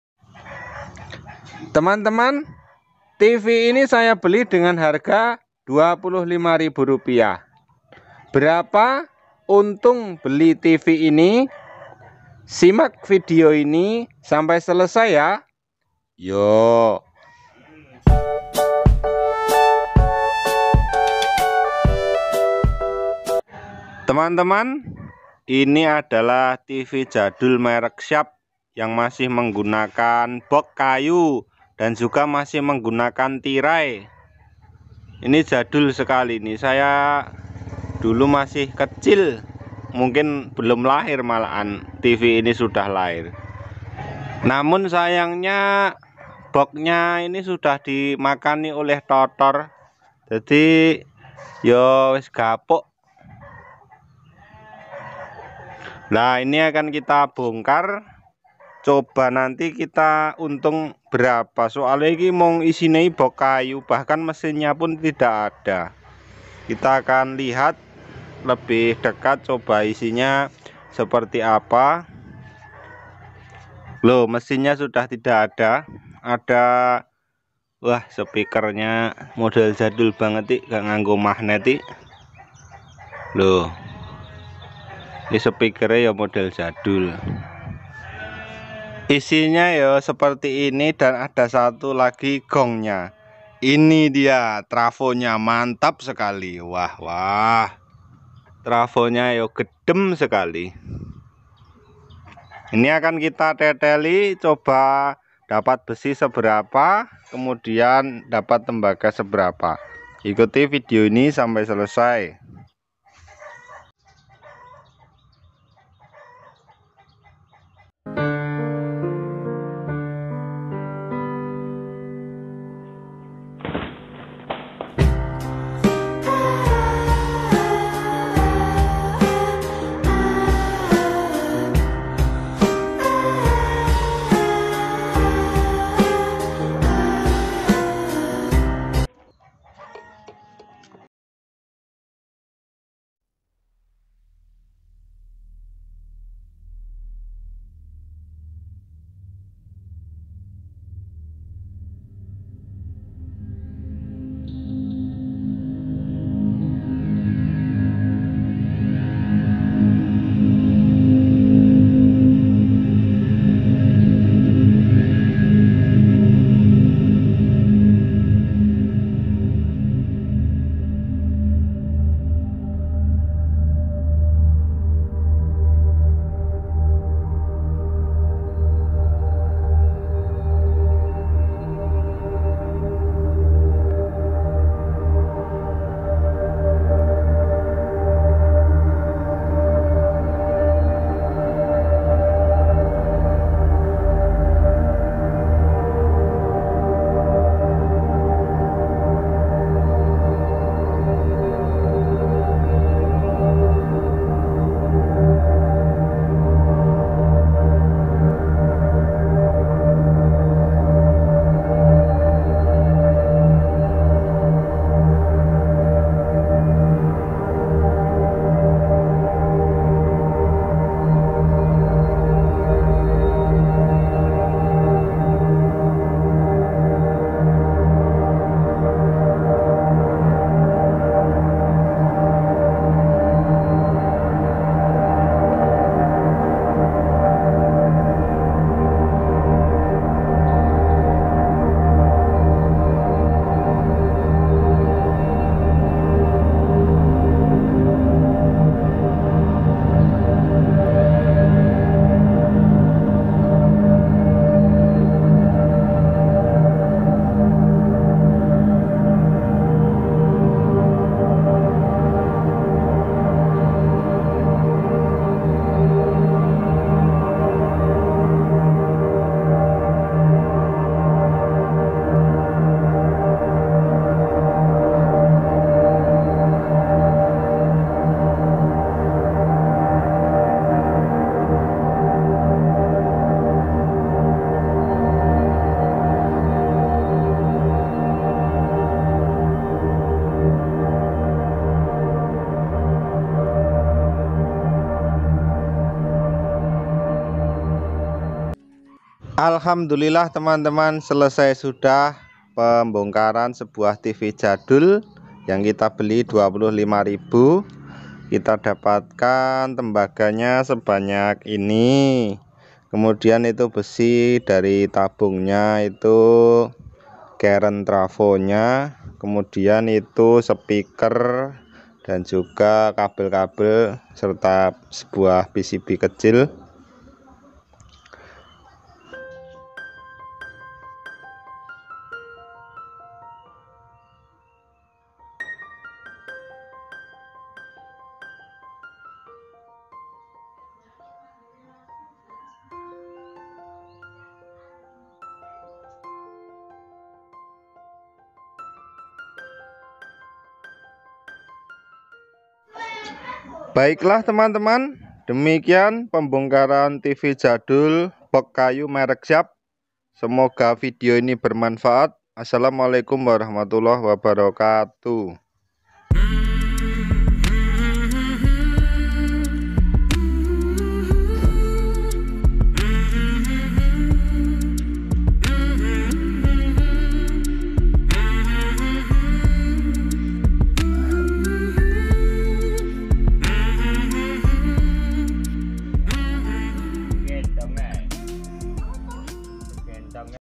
Berapa UNTUNG Beli TV JADUL SHARP ini? Nilai rosok TV jadul.
Kali ini kita membongkar TV jadul
Merek SHARP
Harga beli 25 ribu
Berapa kg besi nya
Berapa kg tembaga nya..
Berapa untung nya..
Yuk simak videonya ya..